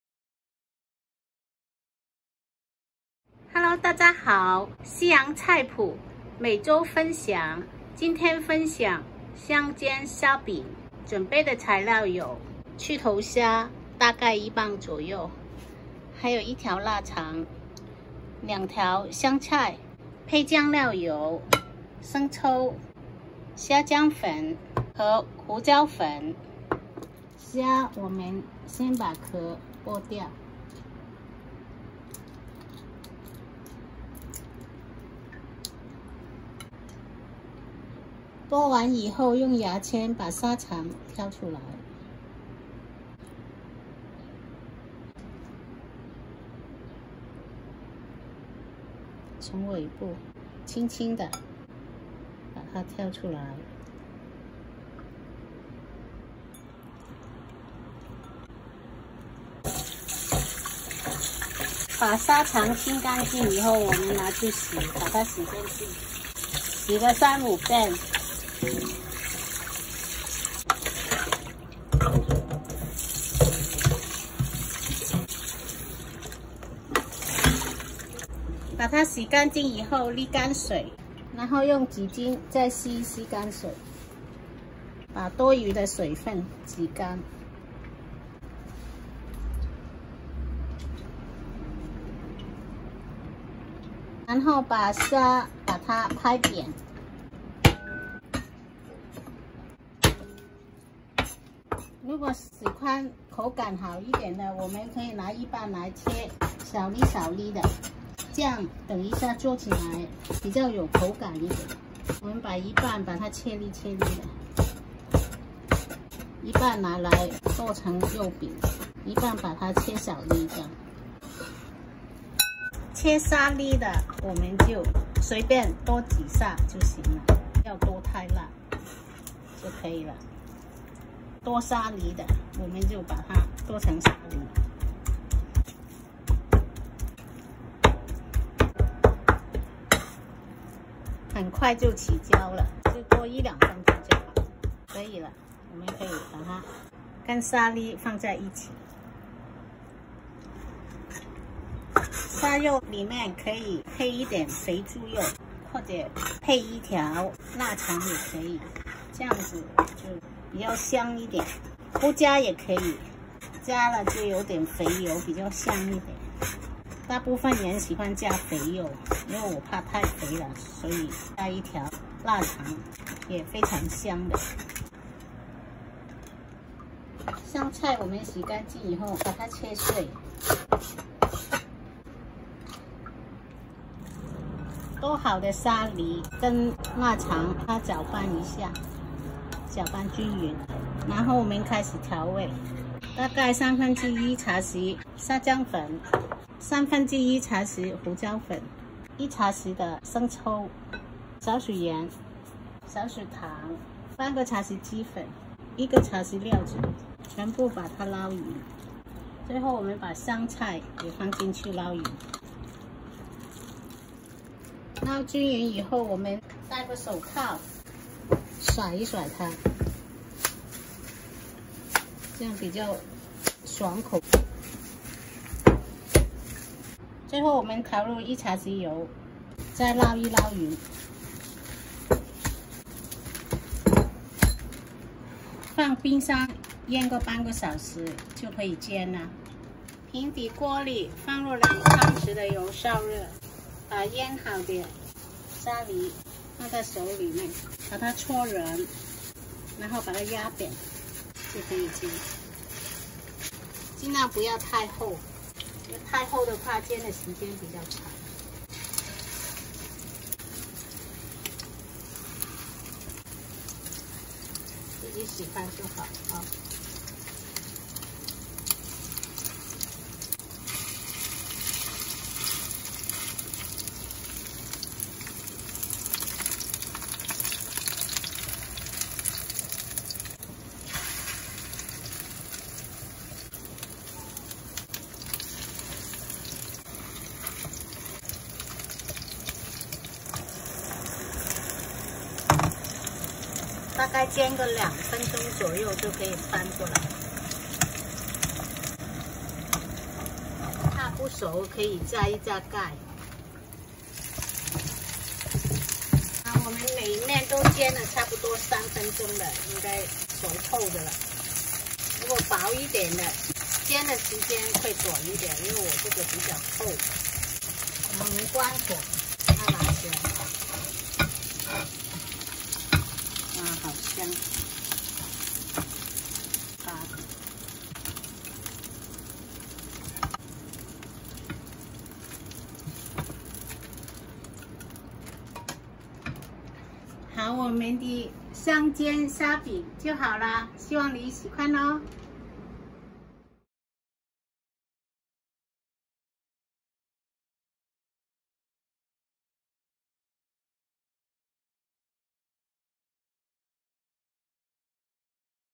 夕陽菜谱: 怎样做"香煎虾饼" How to make fried Shrimp Patties
夕陽菜谱每周分享，今天分享"香煎虾饼"
材料：（大概消费8美元）
1） 去头虾一磅去壳用牙签在虾背后尾部中间挑出虾肠， 洗干净沥干水， 用纸巾吸干水，一半切小粒， 一半拍烂剁成虾泥。剁好的虾泥虾粒放在一起备用。
2） 腊肠一条切碎小丁备用（或者小量肥猪肉切小丁）
3） 香菜两条（或者葱两条）切碎备用
做法： 大碗内放入 1）2）3） 捞均匀， 调入 沙姜粉， 胡椒粉，糖和盐各1/3茶匙， 生抽料酒各一茶匙，鸡粉1/2茶匙， 生粉2茶匙， 全部捞均匀同一方向搅拌一两分钟充分搅拌后，戴上一次性手套， 用手摔打虾泥20下直至感觉虾泥有弹性， 最后调入一茶匙油捞均匀放入冰箱腌20分钟左右备用
大火起锅放入两汤匙油烧热。戴上一次性手套上沾小许油以防沾手。放入适量虾泥，搓圆压扁后放入锅内每面中小火煎两三分钟熟后（如虾饼太厚可以加盖）捞出即可。香煎虾饼，Q弹香口，老少皆宜，下酒又下饭。